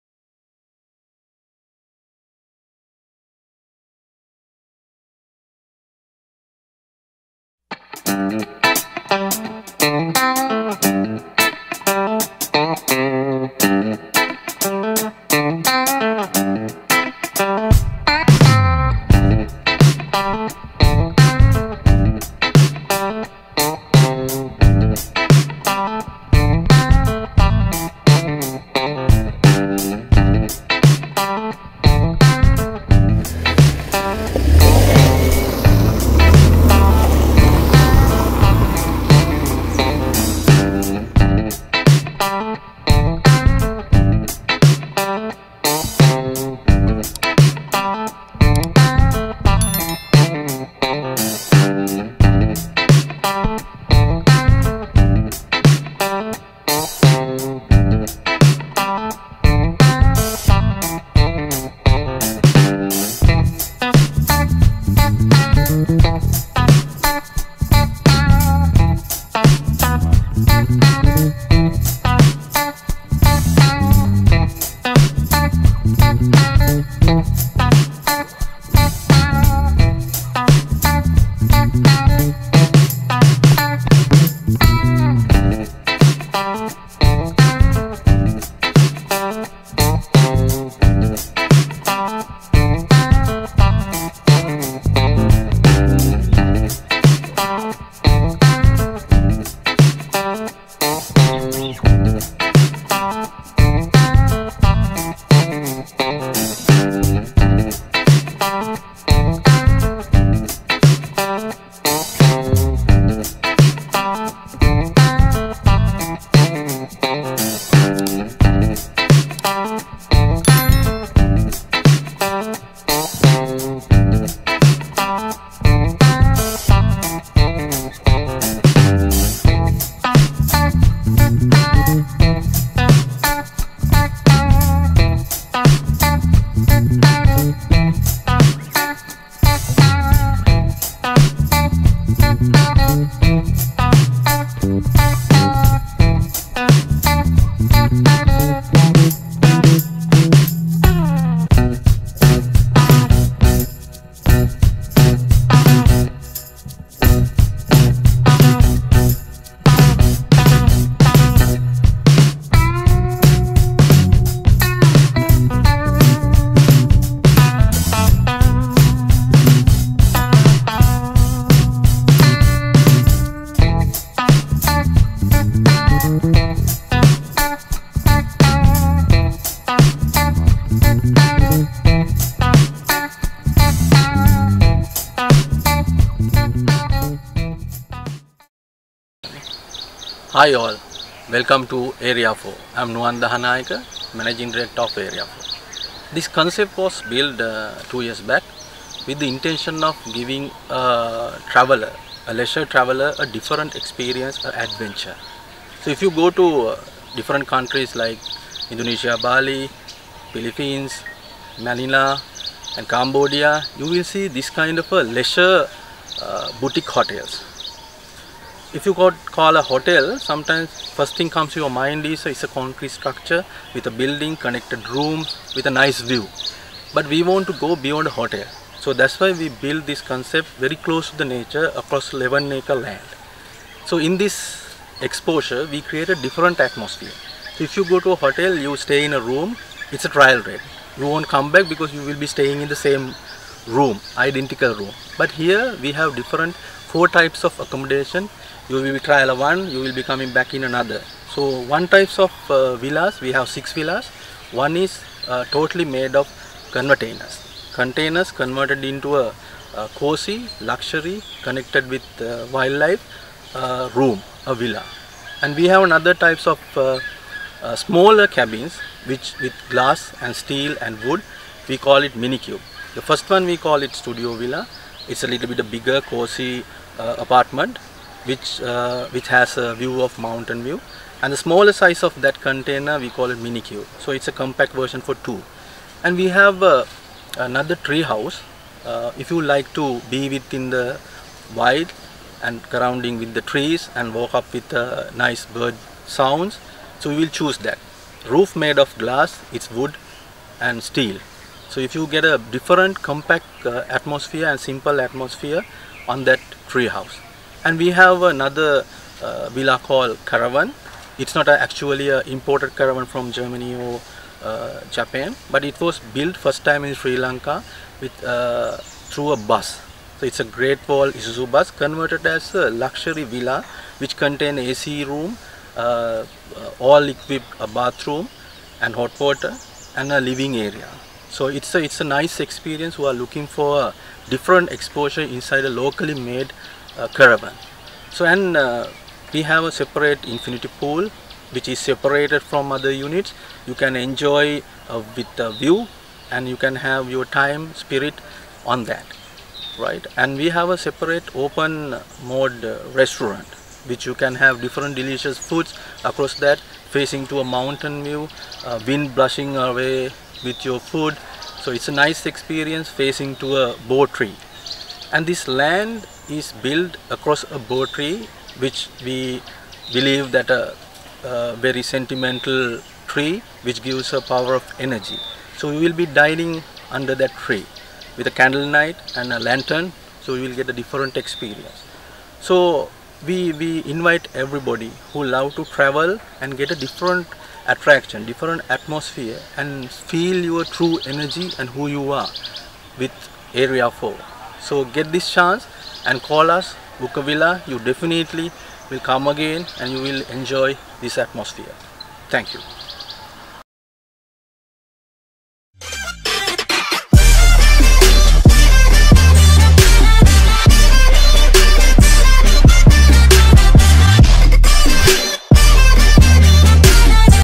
Unique Atmosphere in Ella : Area 4 Eco Cubes
Area 4 is a uniquely designed atmosphere in Ella, spreading over a 11 acre land. There are 4 categories of spacious villas which will ensure your privacy while your stay. These villas are structured with steel, glass and wood where you feel like you are breathing inside the wild.

The location surrounded with misty mountains ⛰, lush greenery 🌱 and a 24 hour soothing breeze, this is a MUST visit destination for you to escape from your busy urban life.

For reservations;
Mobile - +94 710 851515